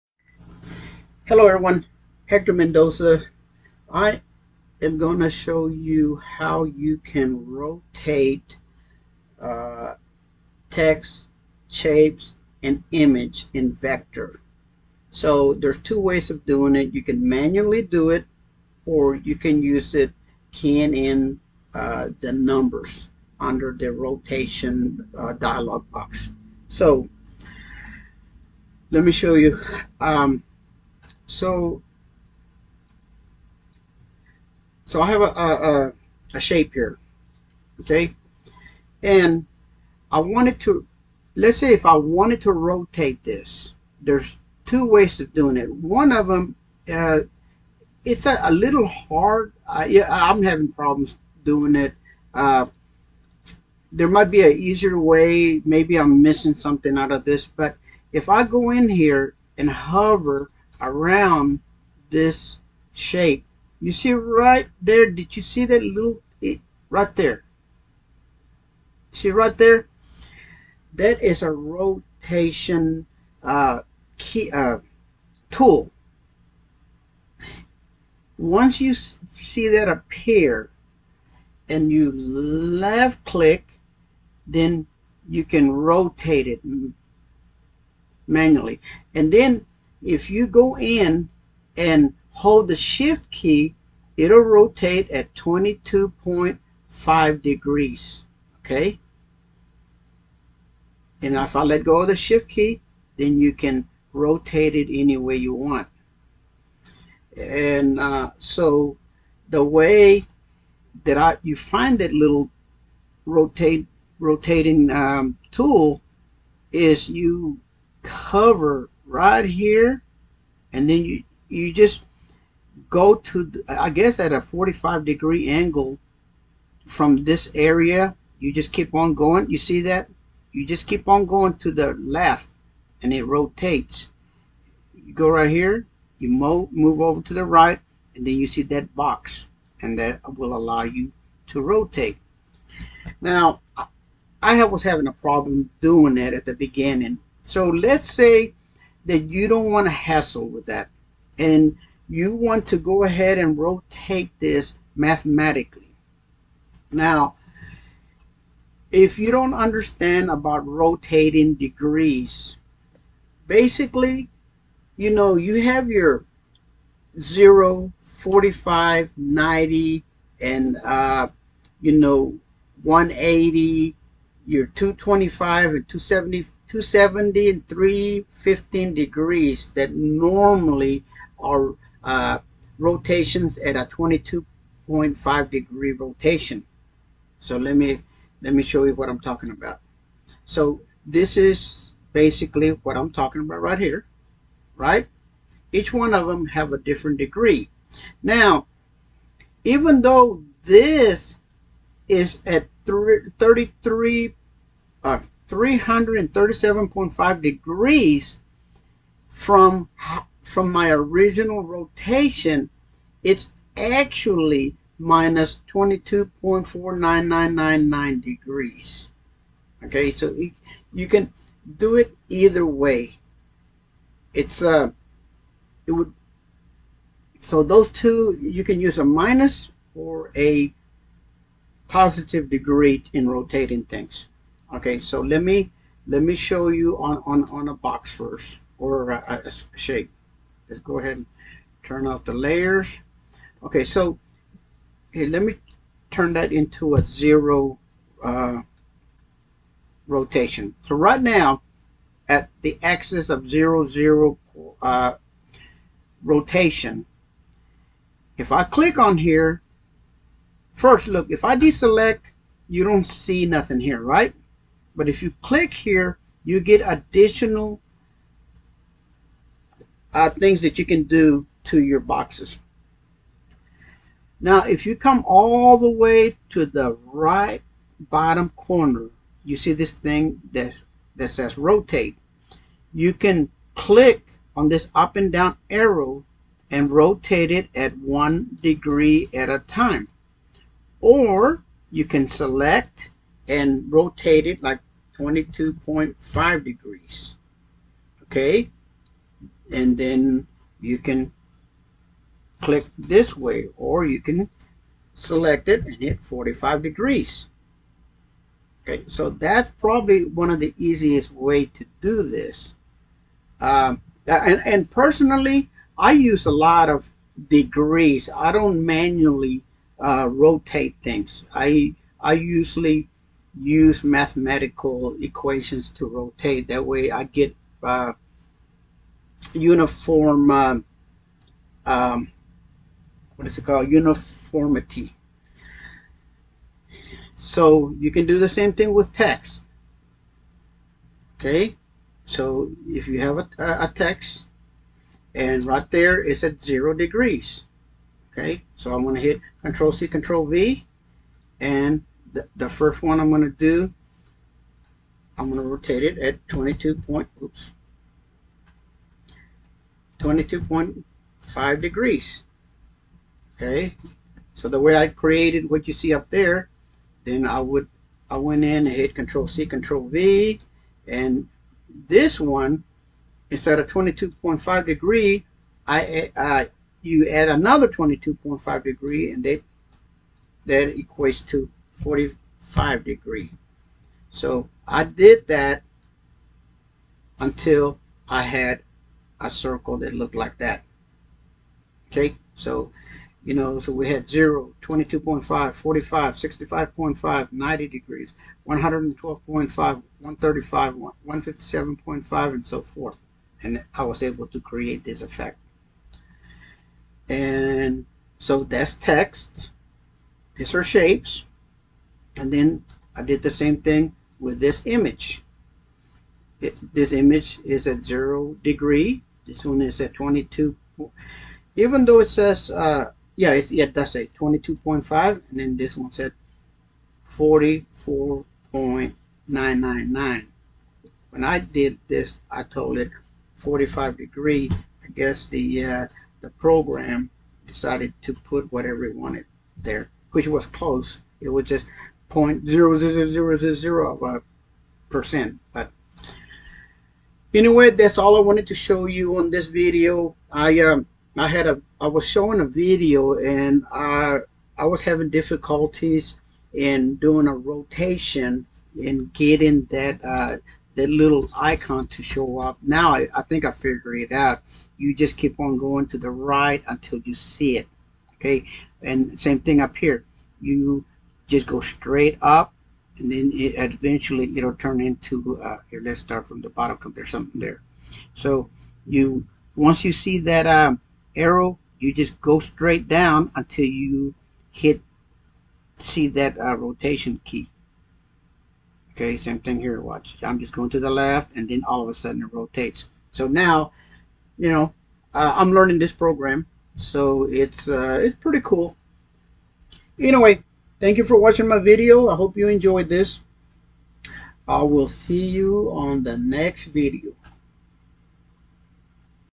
Rotating Text, Shapes, and Images in vectr
Manually rotating text, shapes, and images in vectr

Rotating text, shapes, and image keying in numbers